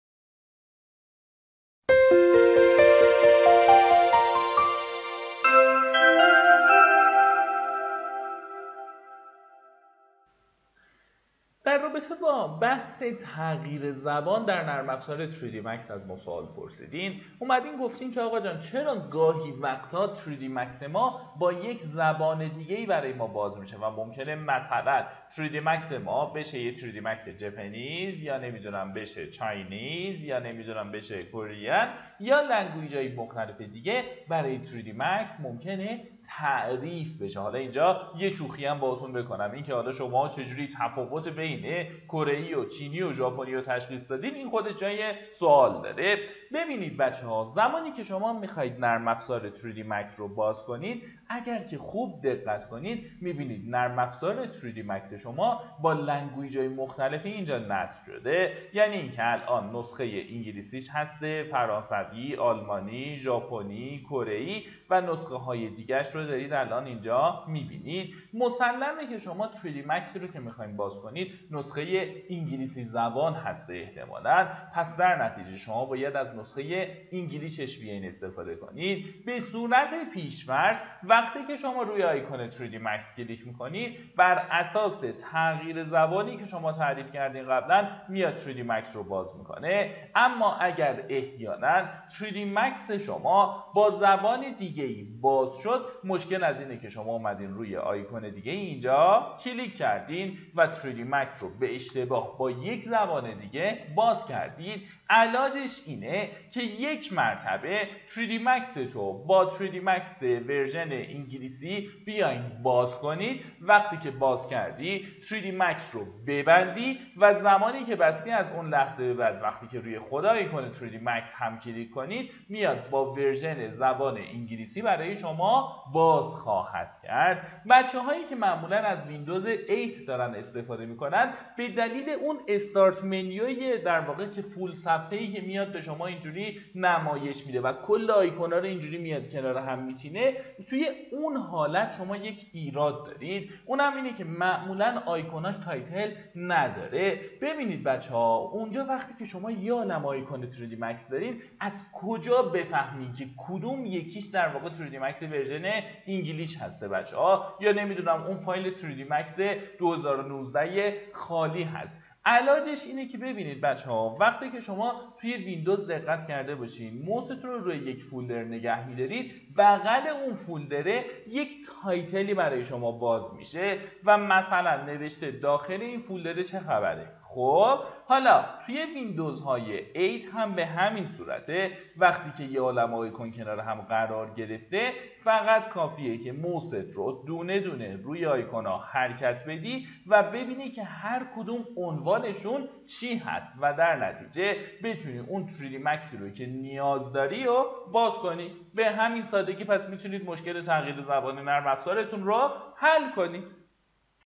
مشکل تغییر زبان در تری دی مکس
مشکل تغییر زبان در تری دی مکس : جواب مشکلات نرم افزاری خود درباره نرم افزارهای ، Autocad ، 3Ds Max Vray و PhotoShop را از ما بپرسید! پاسخ سوال یکی از هنرجویان آرچ آرت در زمینه نرم افزار 3Ds Max برای پرسیدن سوال و دیدن سوالات به سایت ما به نشانی مراجعه کنید.